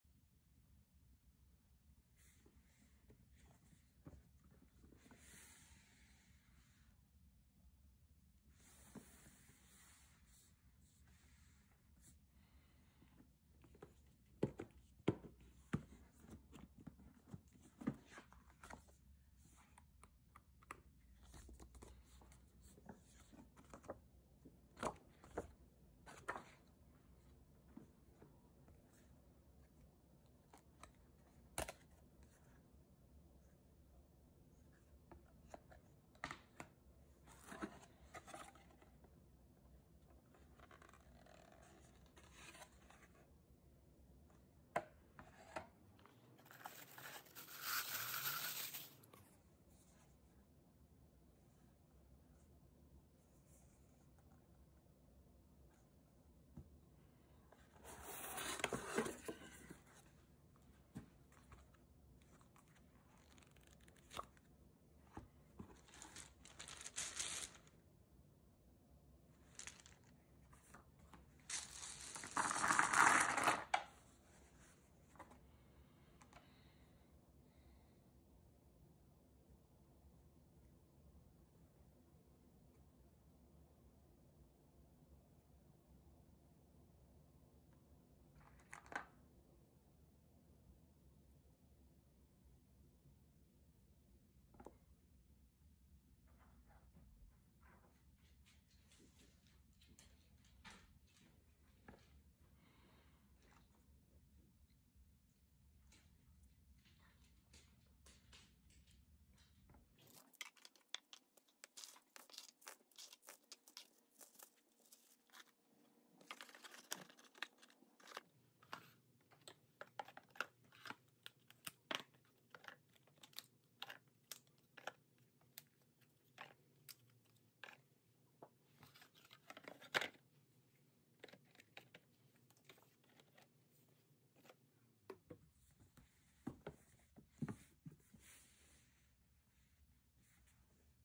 Dice Miner Deluxe Edition Production Sample Unboxing
🎲⛰ Showing off the printer's production sample of Dice Miner’s Deluxe Edition.